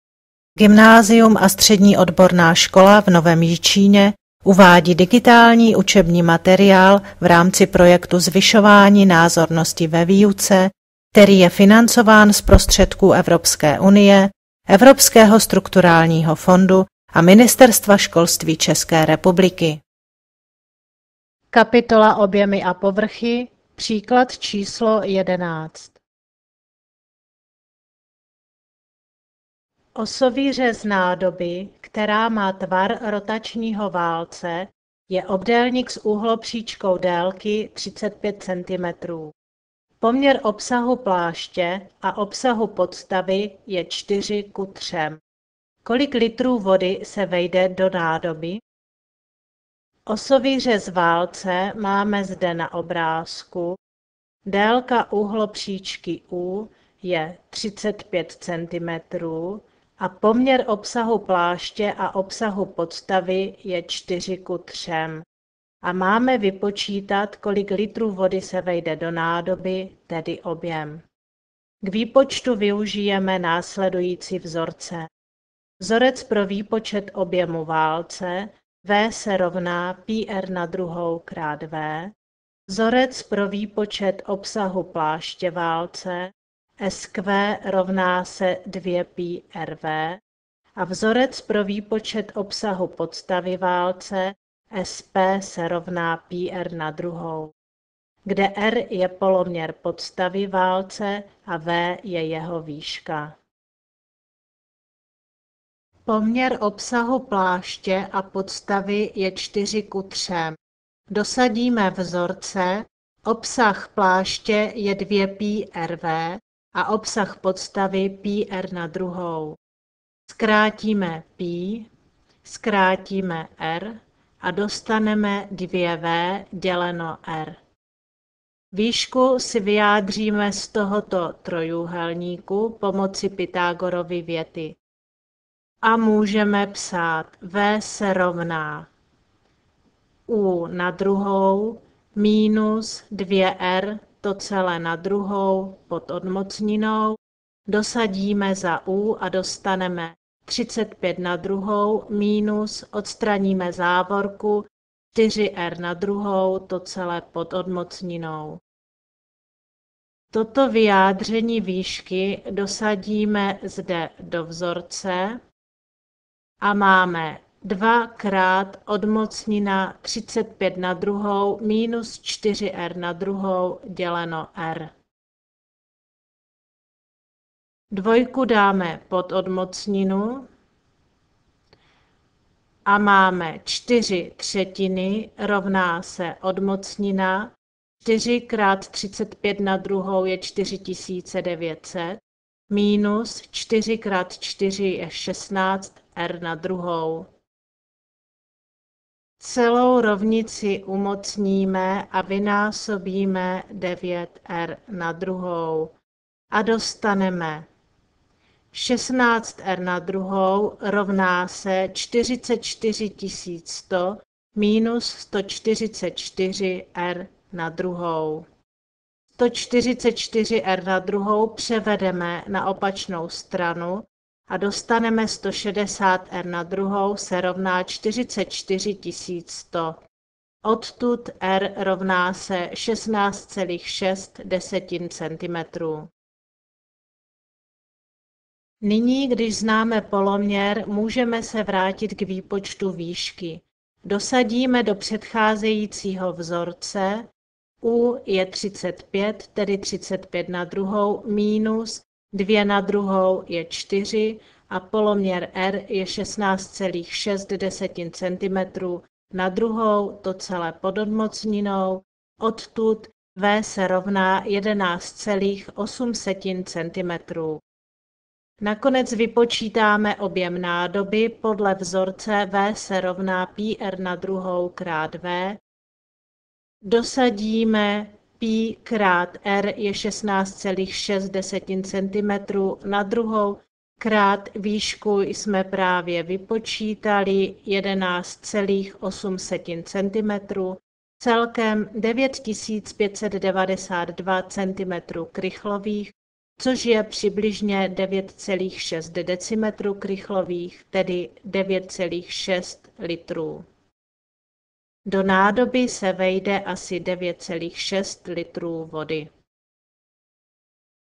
Tu11
Objem rotačního válce a obsah pláště, poměr obsahů -- řešení slovní úlohy